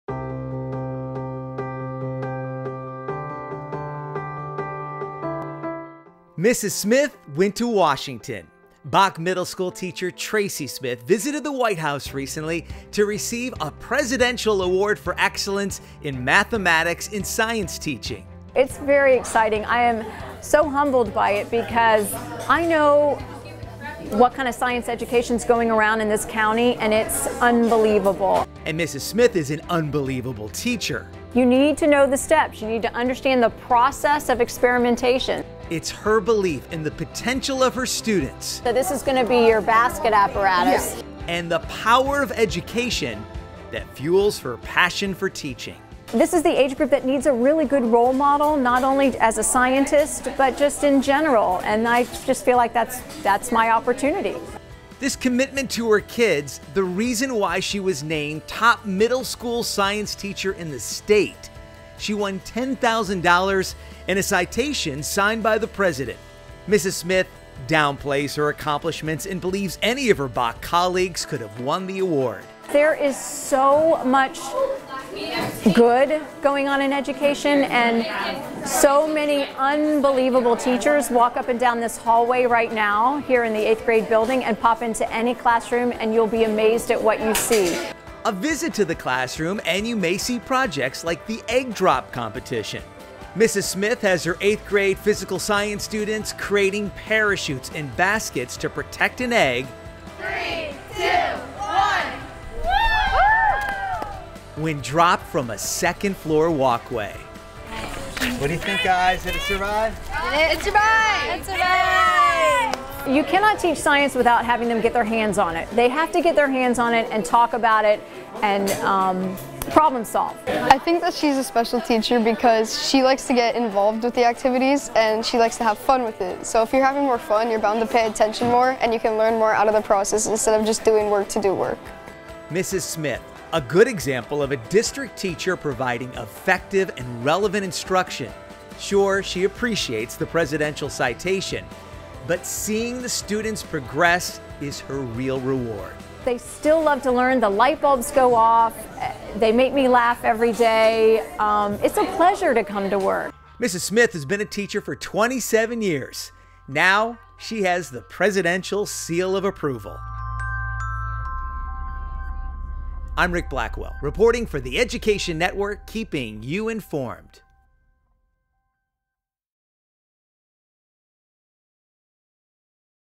Presidential Award for Excellence
BAK Middle School of the Arts science teacher Tracy Smith visited the White House after winning a Presidential Award for Excellence in Mathematics and Science Teaching.